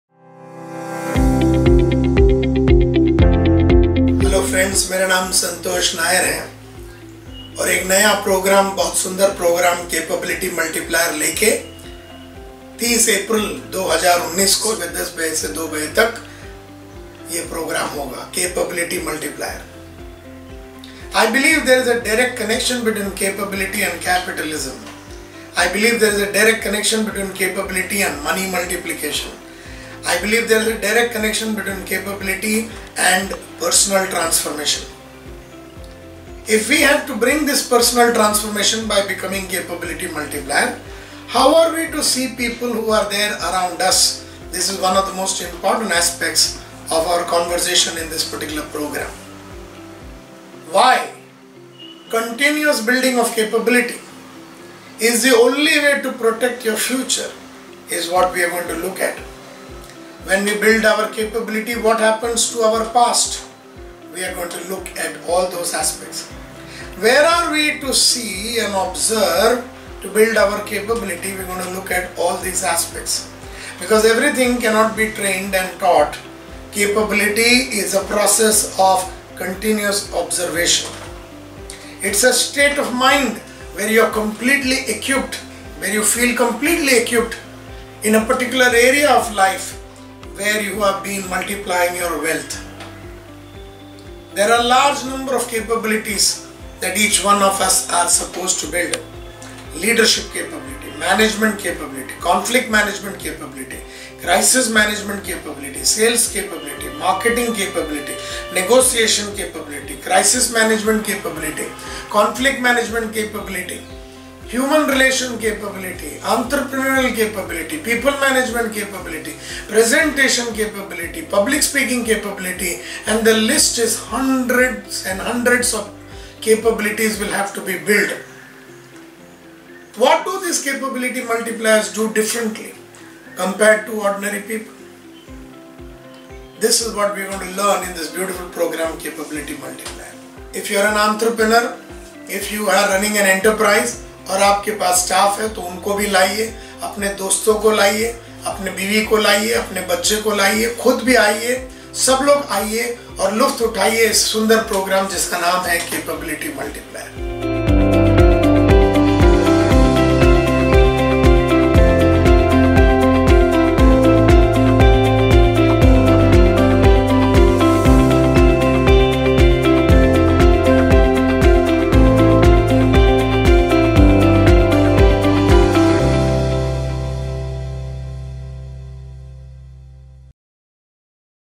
Capability Multiplier | smmart | Santosh Nair | Mumbai
"Come, experience, and witness the power of Capability Multiplication on our lives from the most famous Entrepreneur coach in India, T.I.G.E.R. Santosh Nair. Through the video we get a glimpse of Capability, its advantages, and how by multiplying our capabilities we can achieve the next level of personal, professional success in life which is dearly desired by all of us.

Multiply Capabilities = Multiply success = Multiply quality of life = Multiply happiness"